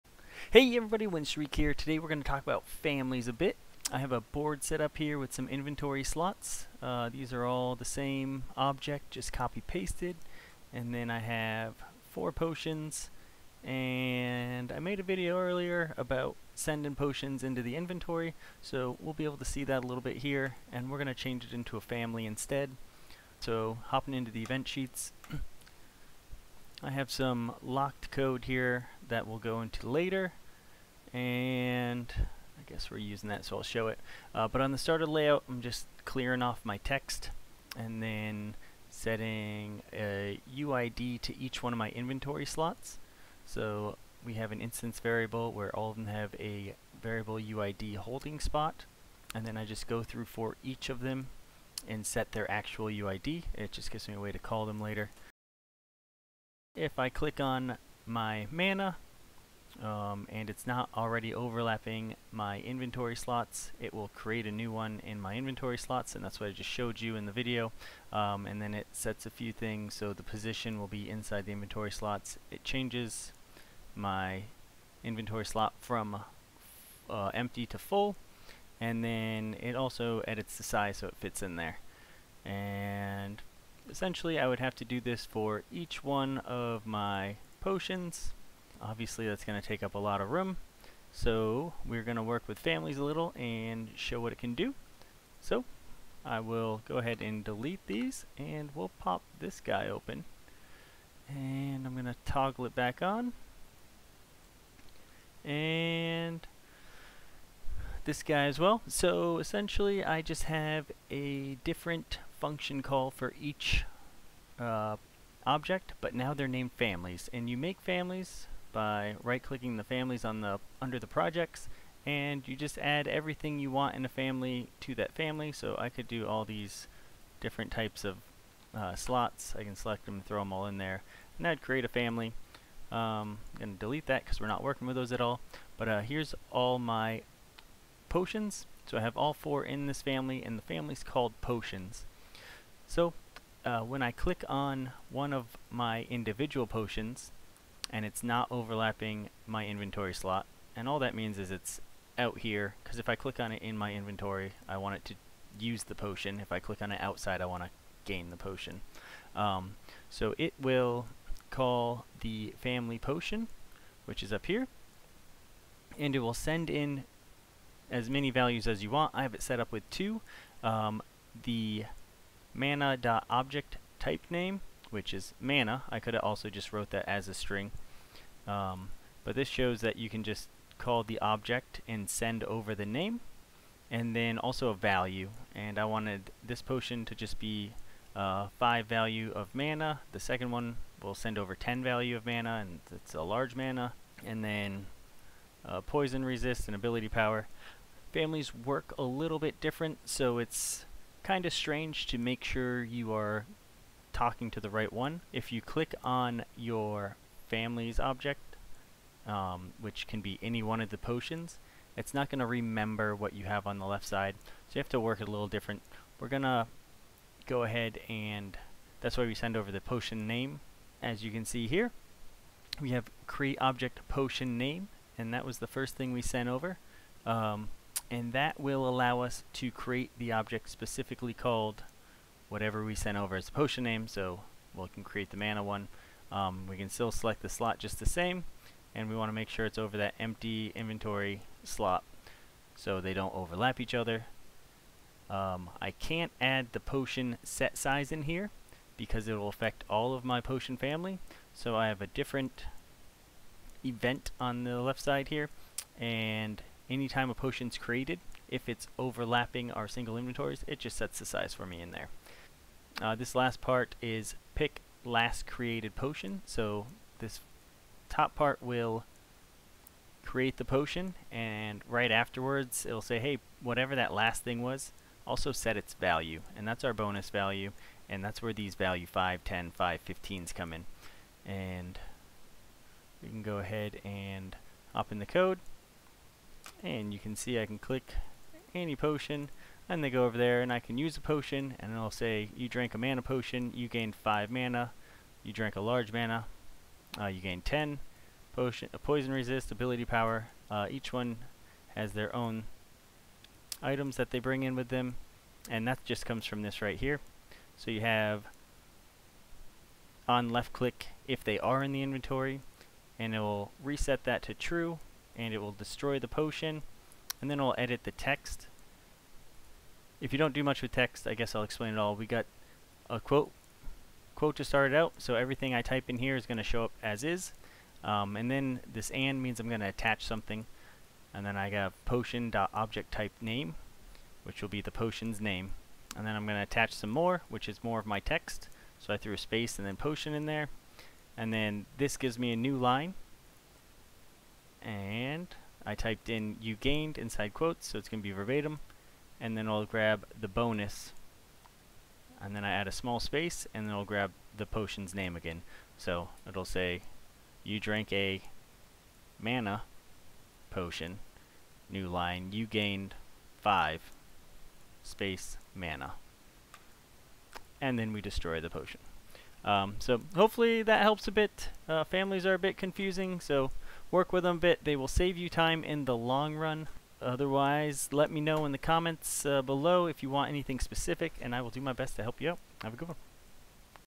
Save time with Families - Construct 3 Tutorials
Save room in your code and headache using Families to group together items that will use similar features.
Graphics:
Potions:
Remaining art:
Myself